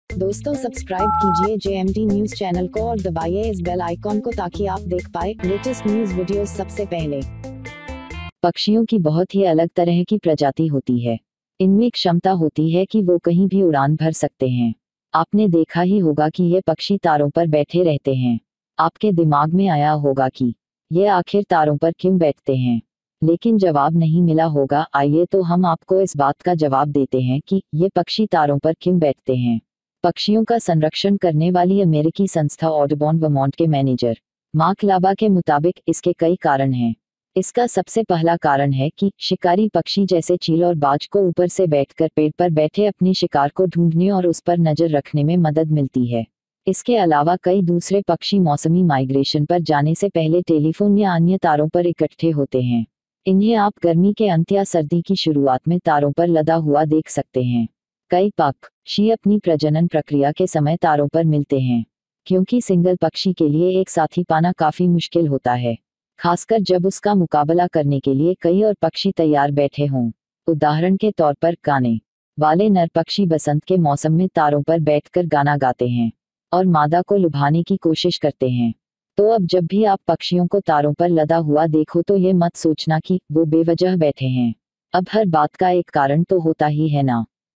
आपने कभी सोचा है कि ये कबूतर, कौआ और चिड़िया तारों पर क्यों बैठते हैं?
आपने कभी सोचा है कि ये कबूतर, कौआ और चिड़िया तारों पर क्यों बैठते हैं?? Jmd News Updates ||

पक्षियों की बहुत ही अलग तरह की प्रजाति होती है इनमें क्षमता होती है कि वो कहीं भी उड़ान भर सकते हैं। आपने देखा ही होगा कि ये पक्षी तारों पर बैठे रहते हैं। आपके दिमाग में आया होगा कि ये आखिर तारों पर क्यों बैठते हैं लेकिन जवाब नहीं मिला होगा, आइए तो हम आपको इस बात का जवाब देते हैं कि ये पक्षी तारों पर क्यों बैठते हैं?पक्षियों का संरक्षण करने वाली अमेरिकी संस्था Audubon Vermont के मैनेजर Mark LaBarr के मुताबिक, इसके कई कारण हैं। इसका सबसे पहला कारण है कि शिकारी पक्षी जैसे चील और बाज को ऊपर से बैठ कर पेड़ पर बैठे अपने शिकार को ढूंढने और उस पर नजर रखने में मदद मिलती है।इसके अलावा कई दूसरे पक्षी मौसमी माइग्रेशन पर जाने से पहले टेलीफोन या अन्य तारों पर इकठ्ठे होते हैं। इन्हें आप गर्मी के अंत या सर्दी की शुरुआत में तारों पर लदा हुआ देख सकते हैं।कई पक्षी अपनी प्रजनन प्रक्रिया के समय तारों पर मिलते हैं। क्योंकि सिंगल पक्षी के लिए एक साथी पाना काफी मुश्किल होता है। खासकर जब उसका मुकाबला करने के लिए कई और पक्षी तैयार बैठे हों। उदाहरण के तौर पर, गाने वाले नर पक्षी बसंत के मौसम में तारों पर बैठ कर गाना गाते हैं और मादा को लुभाने की कोशिश करते हैं।तो अब जब भी आप पक्षियों को तारों पर लदा हुआ देखो तो ये मत सोचना कि वो बेवजह बैठे हैं। अब हर बात का एक कारण तो होता ही है ना!